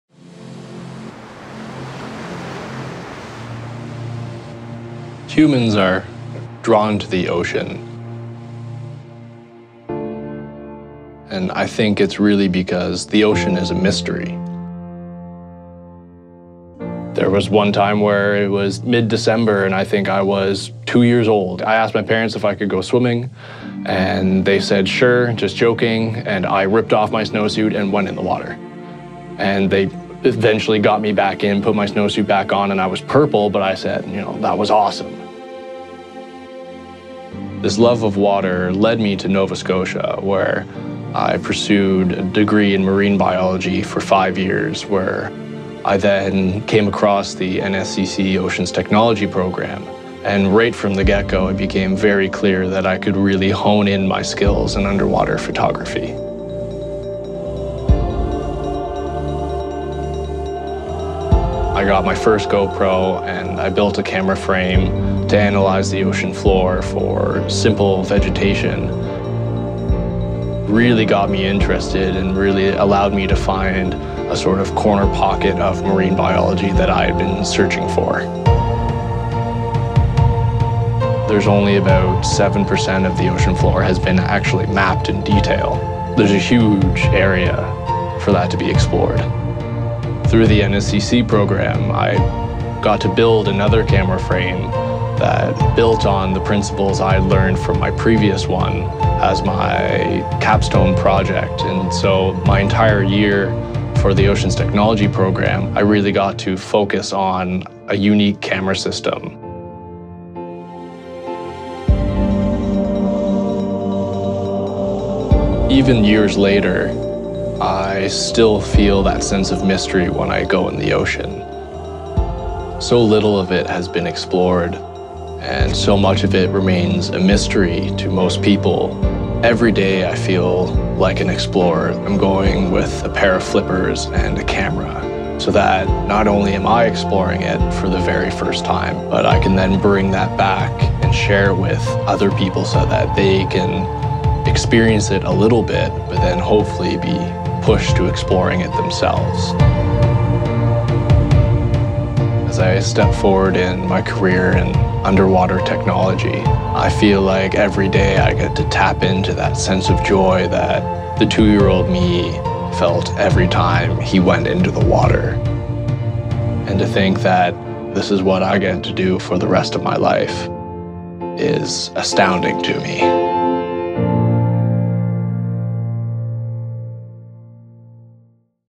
NSCC Strive - Dive In. Make Waves. (Callum Mireault, Oceans Technology)
The mystery of the seas called to Callum from an early age. Now he's working with innovative underwater cameras to explore the depths.